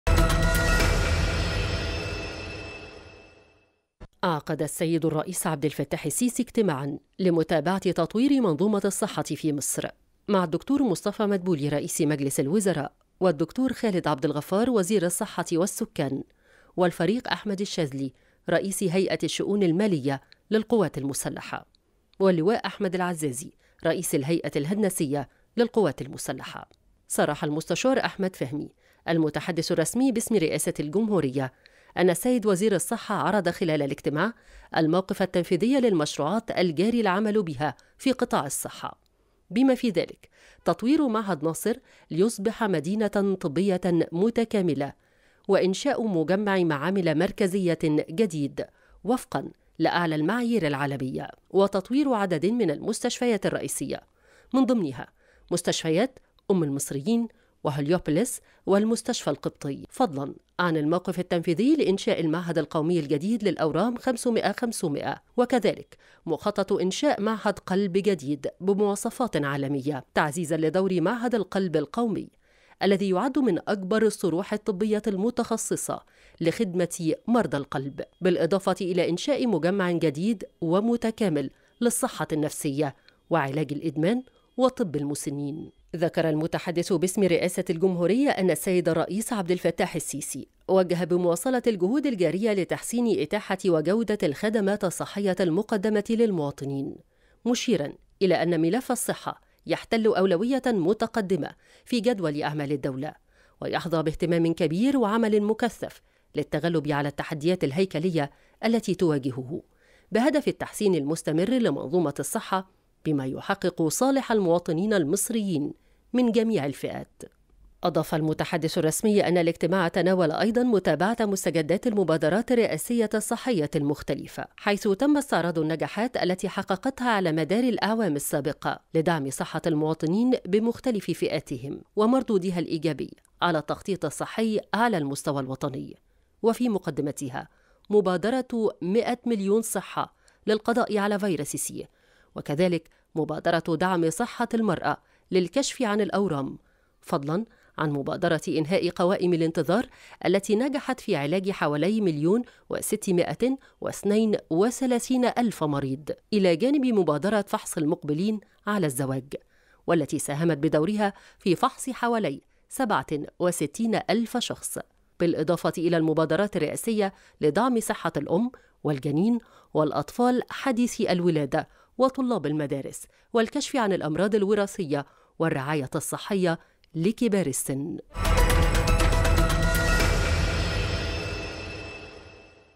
متحدث الرئاسة: الرئيس السيسي يعقد اجتماعا مع رئيس مجلس الوزراء ووزير الصحة وعدد من المسؤولين
تغطية خاصة | متحدث الرئاسة: الرئيس السيسي يعقد اجتماعا مع رئيس مجلس الوزراء ووزير الصحة وعدد من المسؤولين